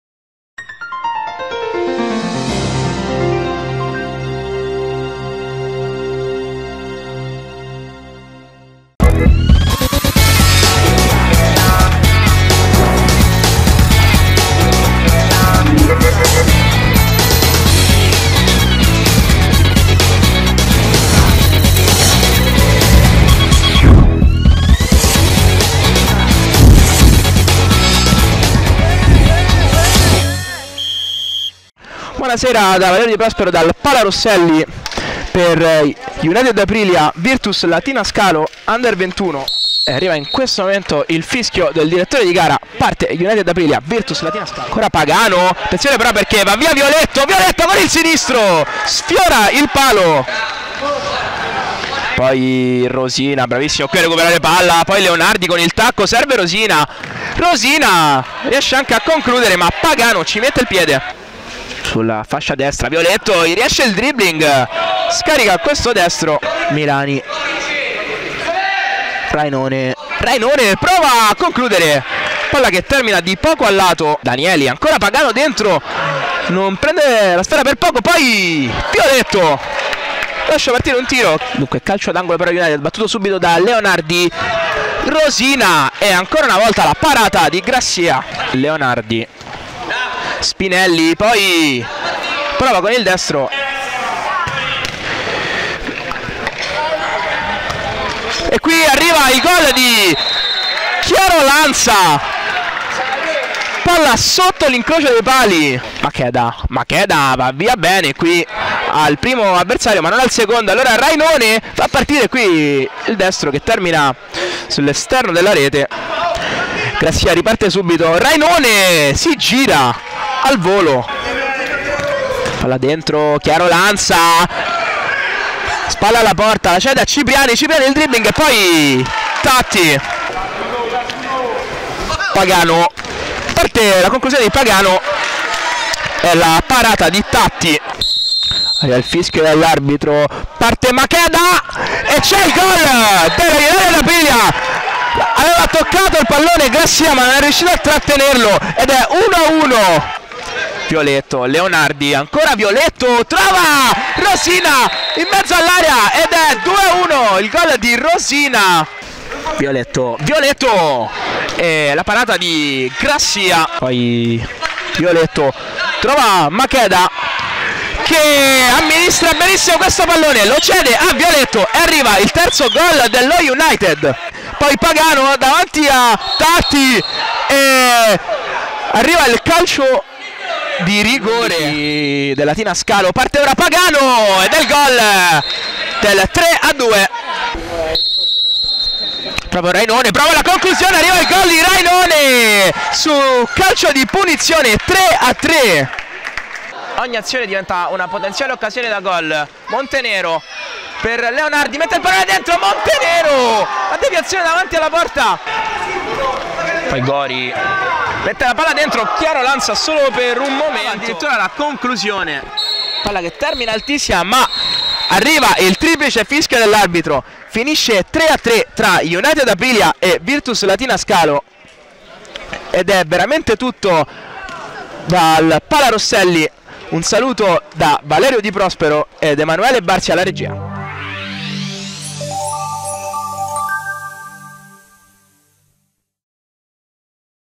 Under 21: United Aprilia vs Virtus Latina Scalo - highlights
Under 21 reg. gir.C, 9a giornata: partita terminata in parità col risultato di 3-3